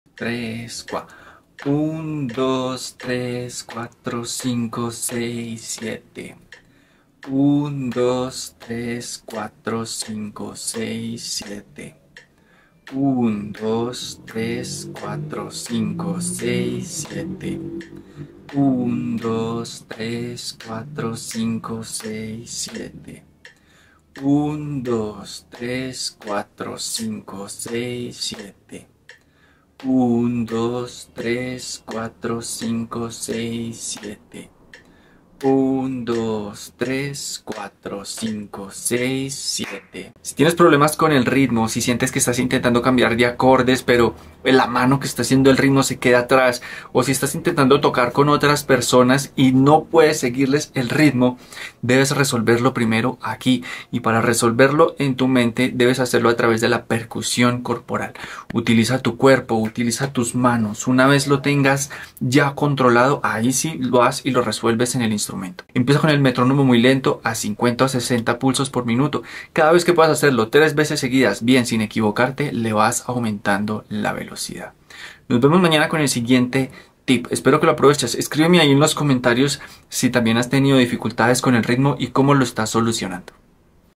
Día 4 🤬 ¿Tienes problemas para seguir el ritmo de una canción? | Camilo Torres
¿Te cuesta coordinar los acordes con la mano rítmica o unirte a un grupo?

No te preocupes más! En este video, vamos a resolver estos desafíos rítmicos con un ejercicio de percusión corporal 🥁.

🔹 Primero, resolvemos el ritmo en la mente: La mejor forma de mejorar tu ritmo es a través de la percusión corporal.

🔹 Ejercicio paso a paso: Te comparto una serie de movimientos con las manos que te ayudarán a mejorar la concentración y coordinación.

🔹 Cuenta los pulsos: Este ritmo consta de 8 pulsos. Es crucial contar mientras realizas los movimientos.

🔹 Comienza lento: Usa un metrónomo a 50-60 pulsos por minuto y aumenta la velocidad gradualmente. Asegúrate de hacerlo al menos 3 veces sin equivocarte antes de subir la velocidad.

🔹 Desafíos rítmicos: Recuerda, el ritmo debe resolverse primero en la mente y luego en el instrumento.

¡Si no quieres ser el último en terminar la canción, aprende estos 7 ejercicios! 🥁

¡Nos vemos mañana en el próximo Tip! 🎶